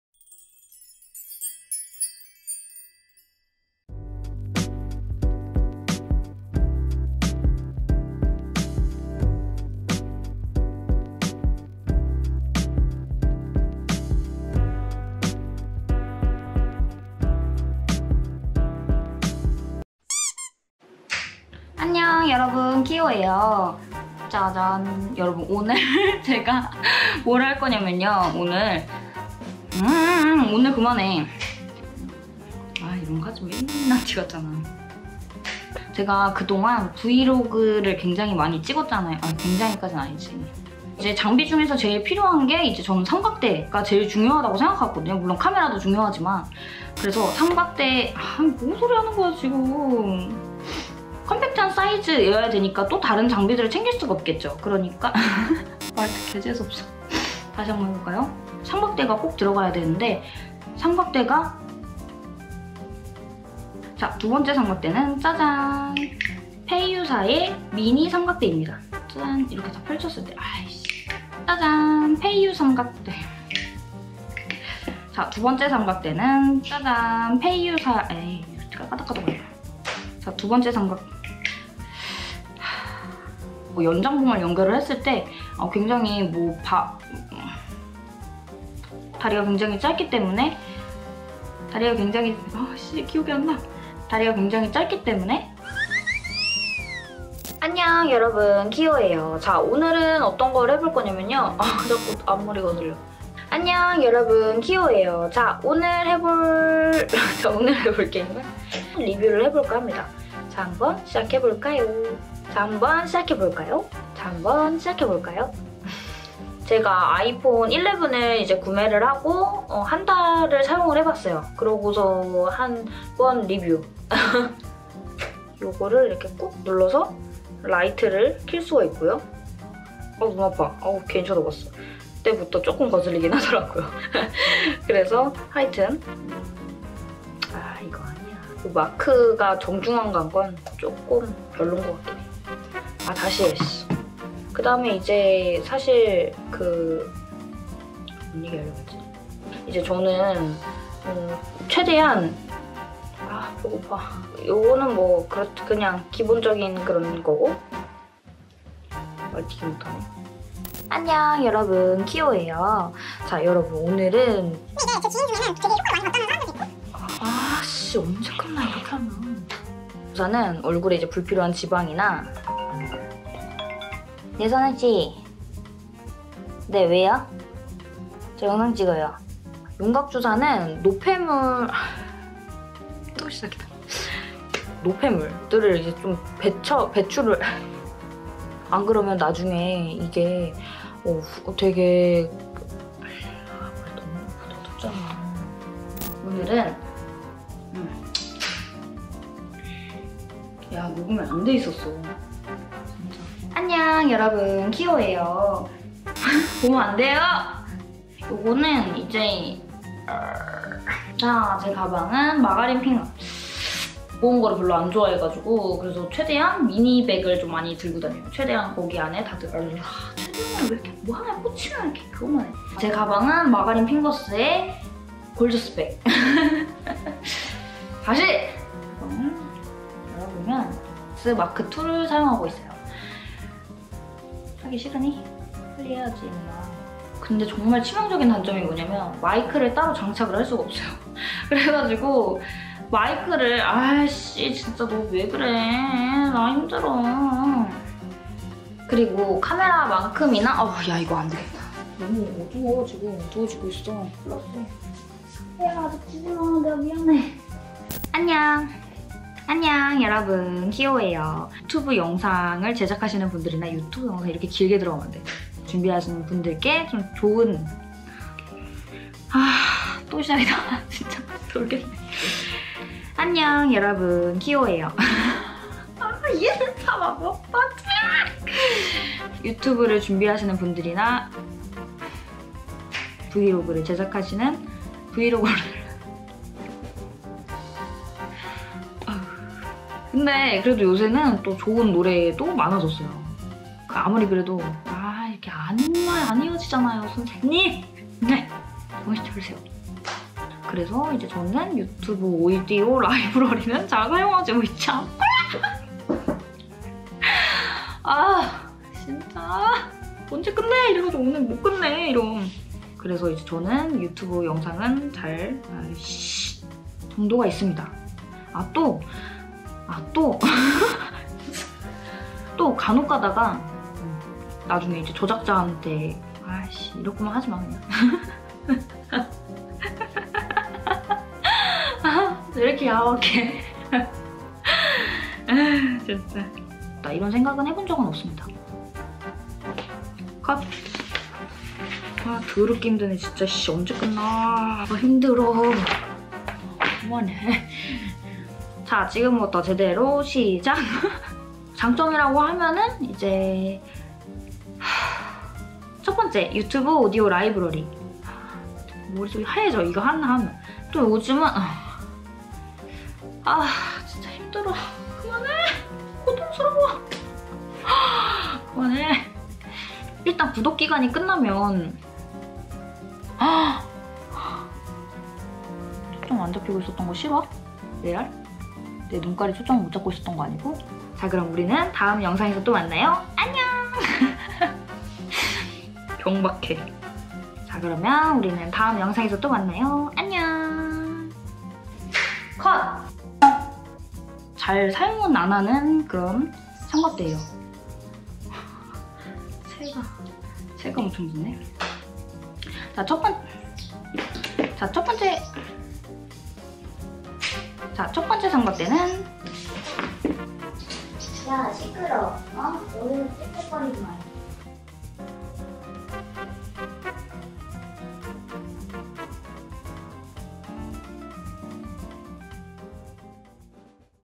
대환장 분노의 리뷰 NG 모음 ※혼잣말 주의😂※
안녕하세요 키오입니다~!
오늘은 그동안 모아놨던 리뷰 영상의 자투리 NG 모음을 가져왔습니다!!
재미있게 봐주세요 :D